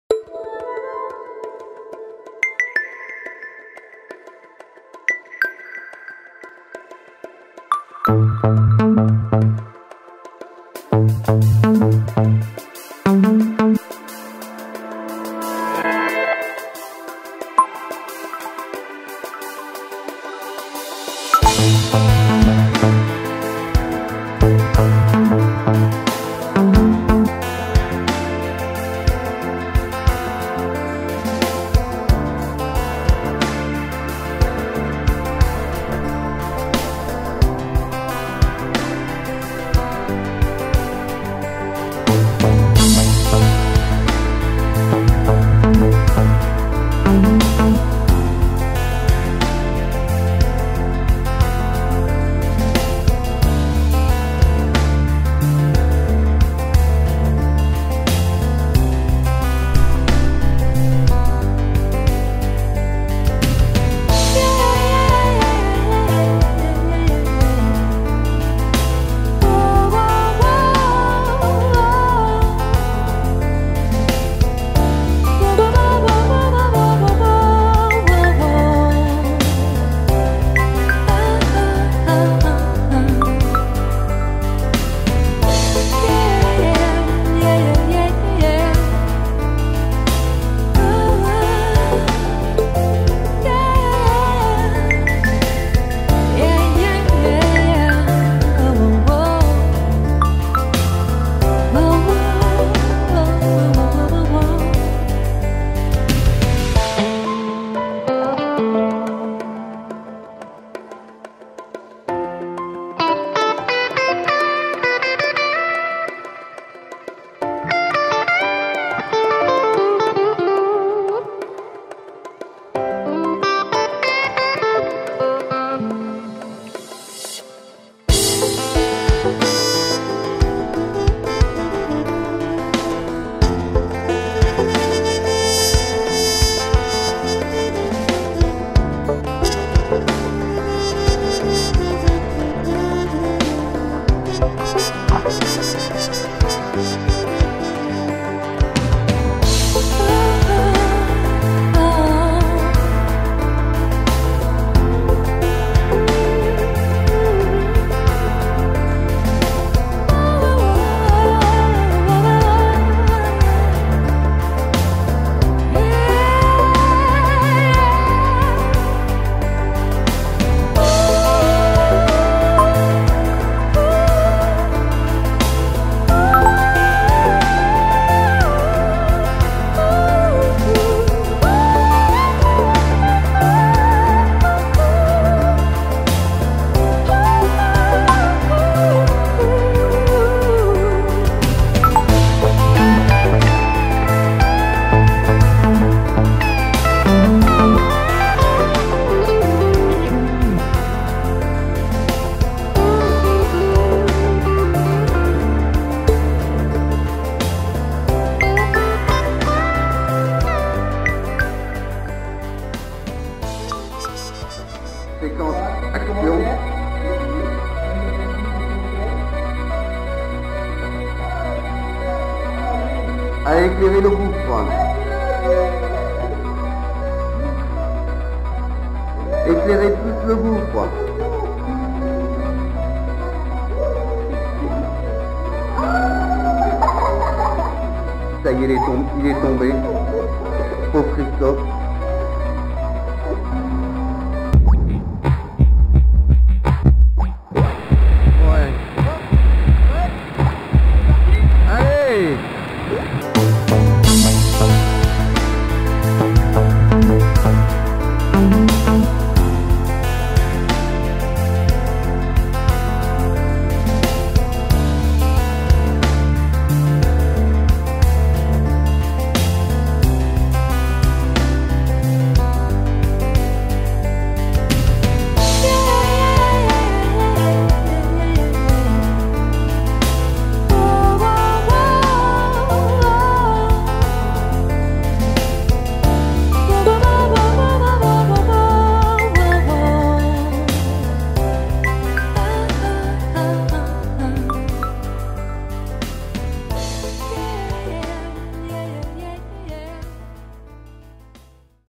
OCCITANIE/KAYAK PADDLE SPELEO/ BASE NAUTIQUE DE LOTAVENTURE/ LOT 46/ Cajarc
Canoë kayak paddle,spéléologie,grotte acrobatique,escalade,canyoning, parcours aventure en falaise avec Lotaventure à la base nautique de Cajarc/Saint Cirq Lapopie/Lot/46/France/Occitanie.